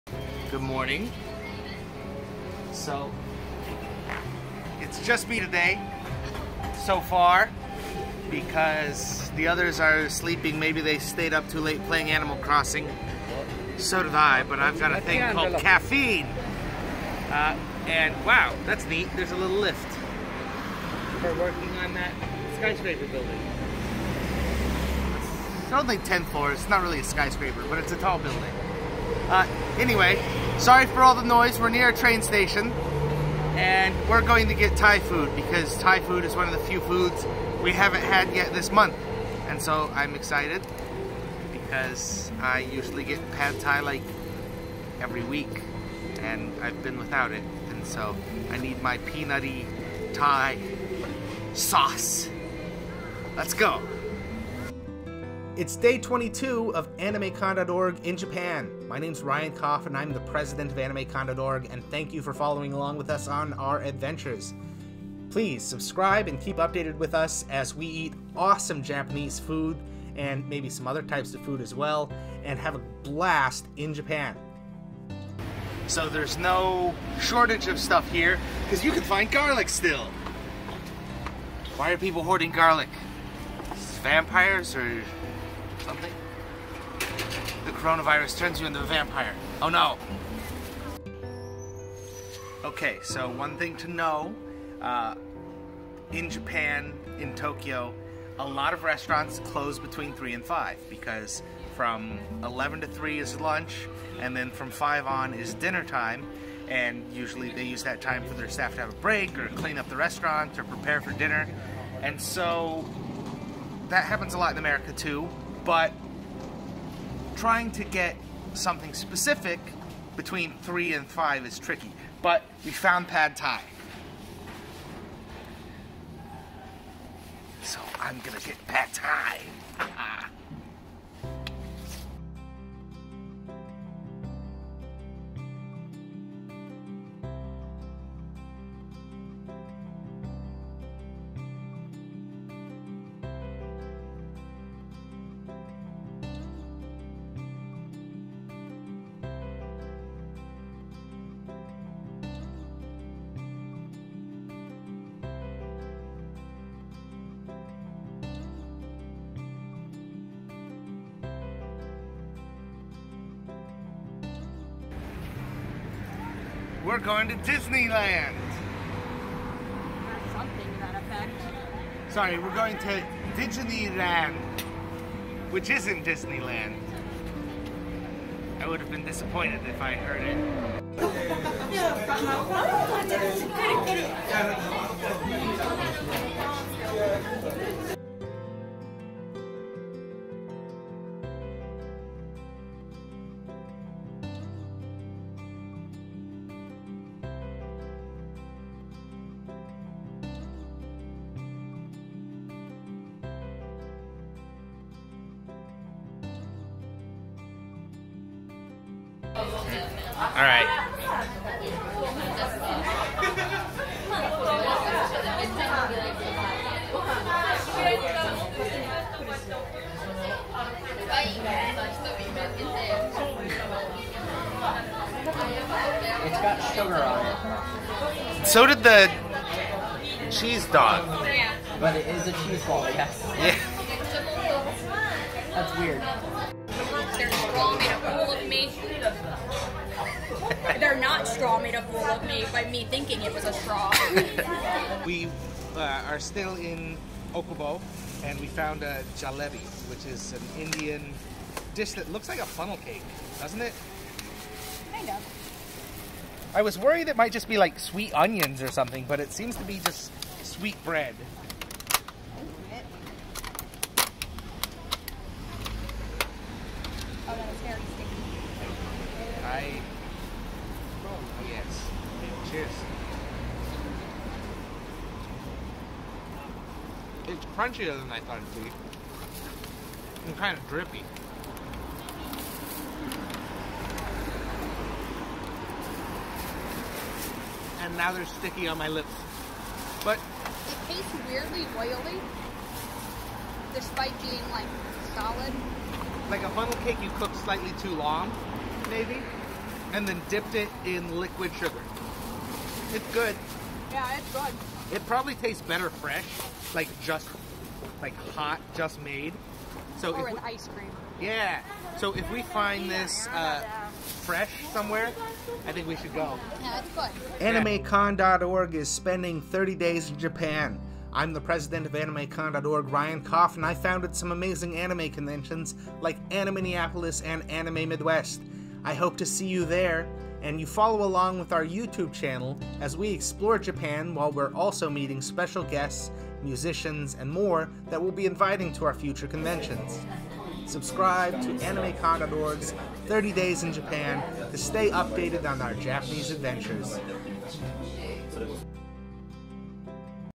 Day 22 of 30 days in Japan! Eating foods in Shin-okubo and Okubo, in Tokyo.
AnimeCon.org is excited to bring you footage from Japan. Today we are eating foods in Shin-okubo and Okubo, in Tokyo.

PS Do you want to learn Japanese? Read this!

* PAD THAI - I love Pad Thai, and I have to adventure to get some! It's a little tough to find a place that's open between 3pm-5pm (most places are just between lunch and dinner), but I find it!

* Melty Cheese and Chicken - This is something that has taken all over the Okubo area. Honestly, it wasn't the greatest. But you dip wings in the melty cheese!